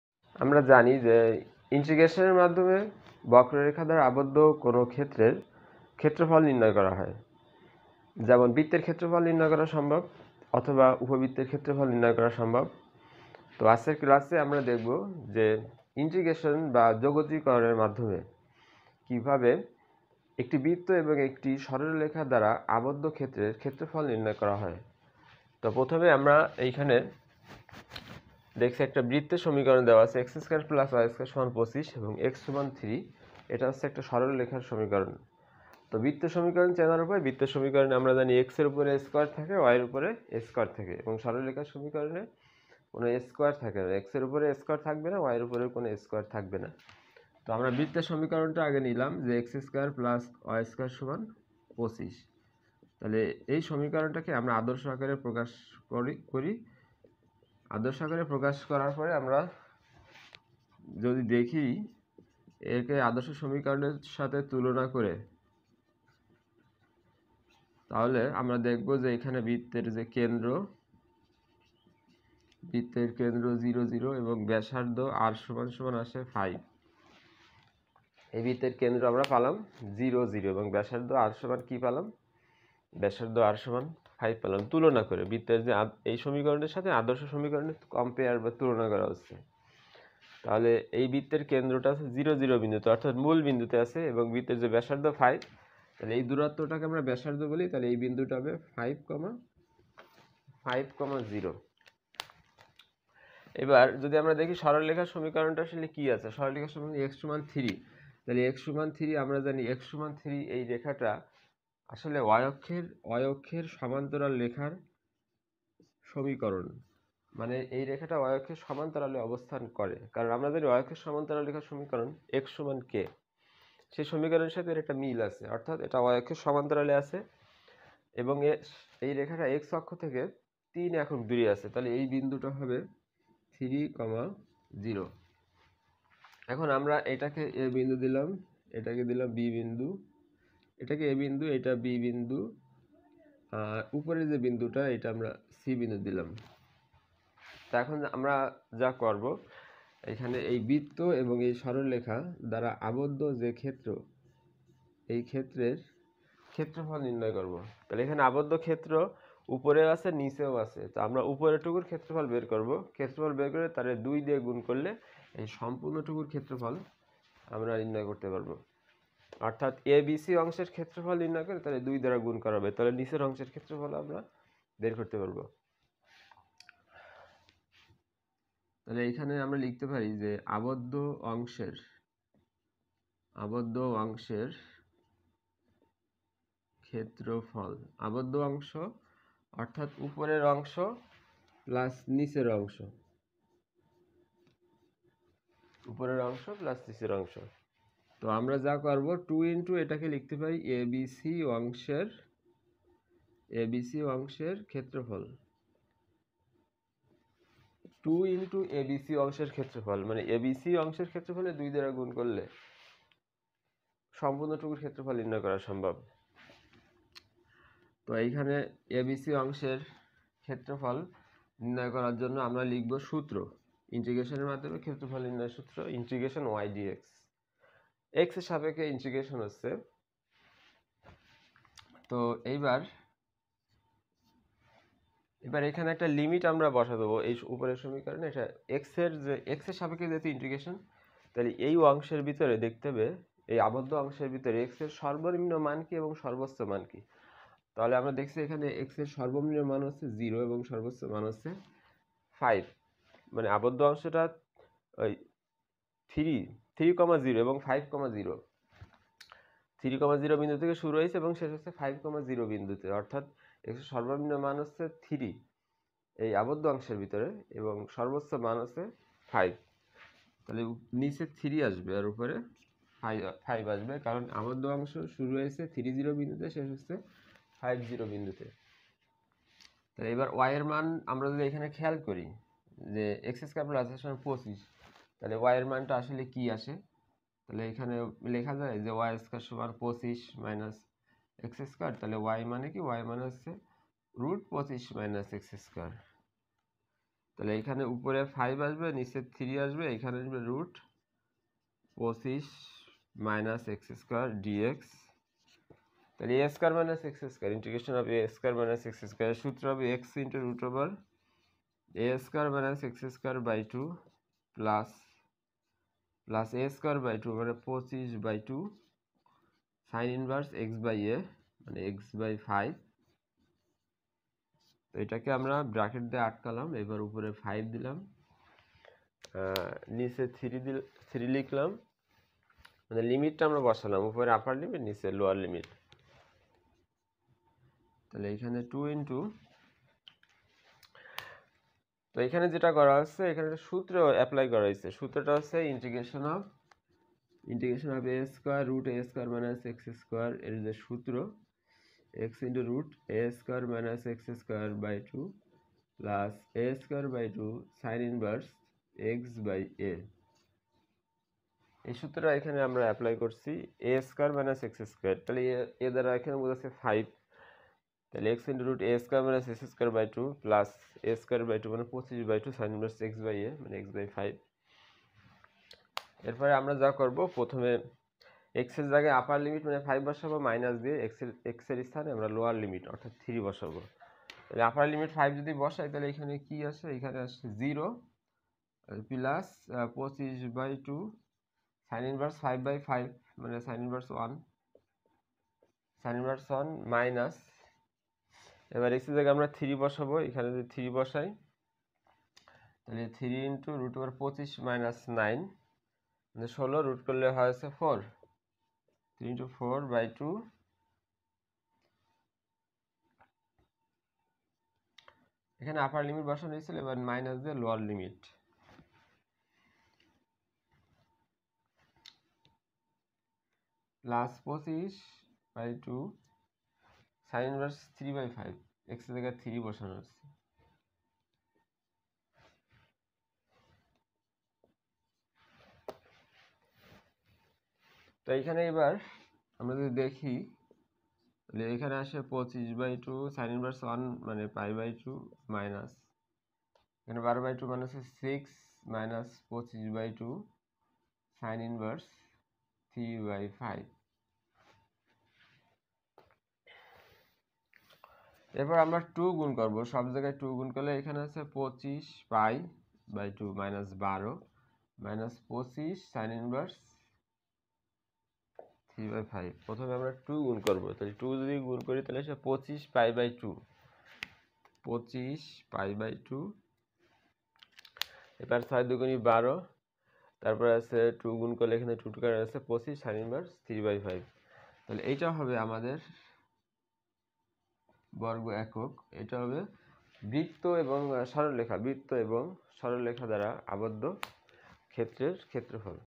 যোগজীকরণের মাধ্যমে বৃত্ত ও সরলরেখা দ্বারা আবদ্ধ অংশের ক্ষেত্রফল নির্ণয়। যোগজীকরণ:৩। উচ্চতর গণিত।
In this video I will describe you  integration.